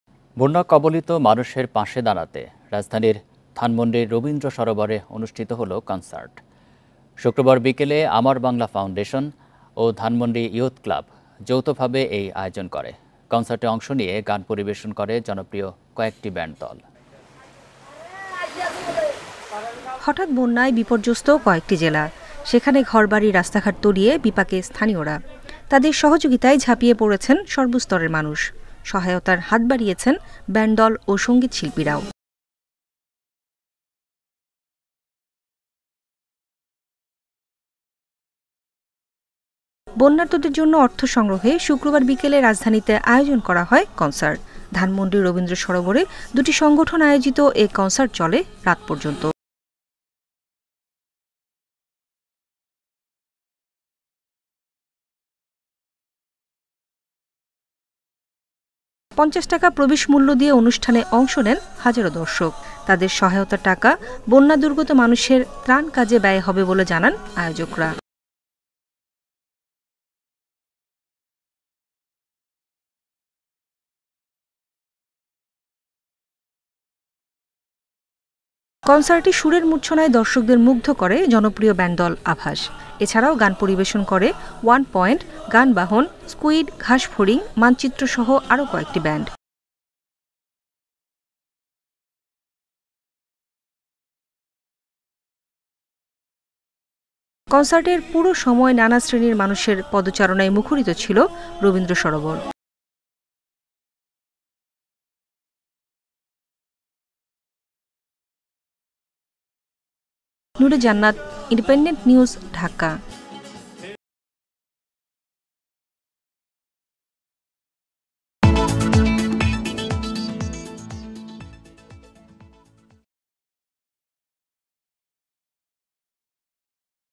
বন্যার্তদের সাহায্যে ধানমন্ডির রবীন্দ্র সরোবরে কনসার্ট । FLOOD | CONCERT | Independent TV
বন্যাকবলিত মানুষের পাশে দাঁড়াতে রাজধানীর ধানমন্ডির রবীন্দ্র সরোবরে অনুষ্ঠিত হলো কনসার্ট। শুক্রবার বিকেলে আমার বাংলা ফাউন্ডেশন ও ধানমন্ডি ইয়ুথ ক্লাব যৌথভাবে এই আয়োজন করে। কনসার্টে অংশ নিয়ে গান পরিবেশন করে জনপ্রিয় কয়েকটি ব্যান্ড দল|

A concert was held at Rabindra Sarovar in Dhanmondi of the capital to stand by the flood-affected people. Amar Bangla Foundation and Dhanmondi Youth Club jointly organized this on Friday afternoon. Some popular bands participated in the concert and performed songs.